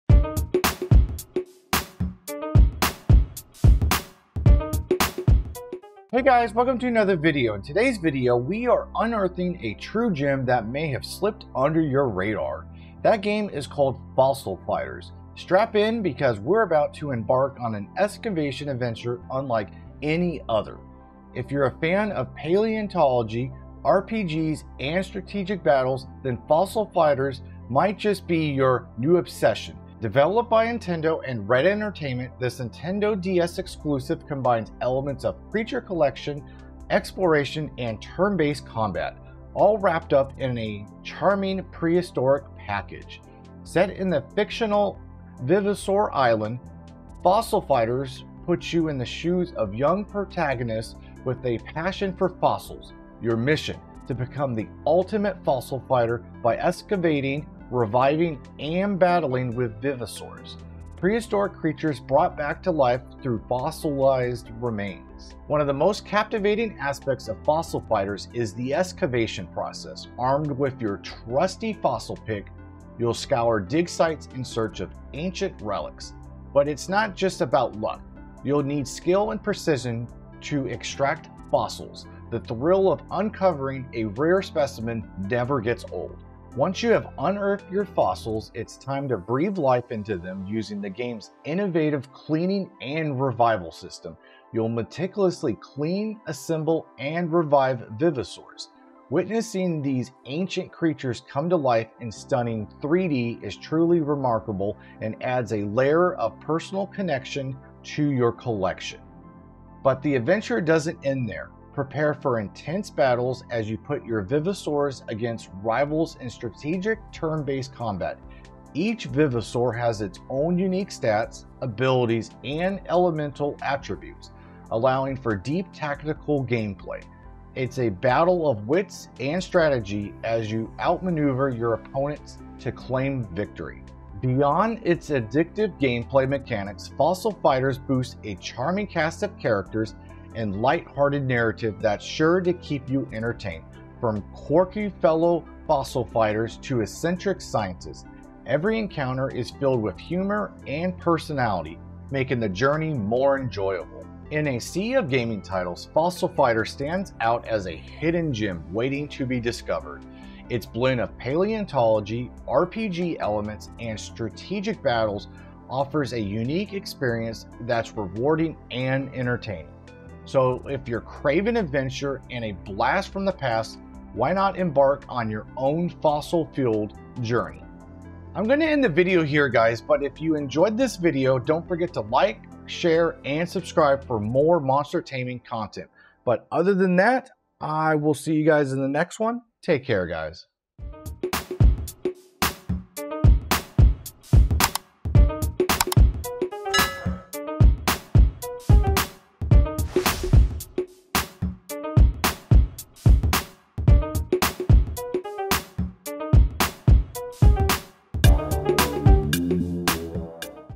Monster Taming Hidden Gems | Fossil Fighters
The Most Underrated Game Ever

Today, we're unearthing a true gem that may have slipped under your radar. That game is called Fossil Fighters. Strap in because we're about to embark on an excavation adventure unlike any other!

If you're a fan of paleontology, RPGs, and strategic battles, then Fossil Fighters might just be your new obsession. Developed by Nintendo and Red Entertainment, this Nintendo DS exclusive combines elements of creature collection, exploration, and turn-based combat, all wrapped up in a charming prehistoric package.

Hastags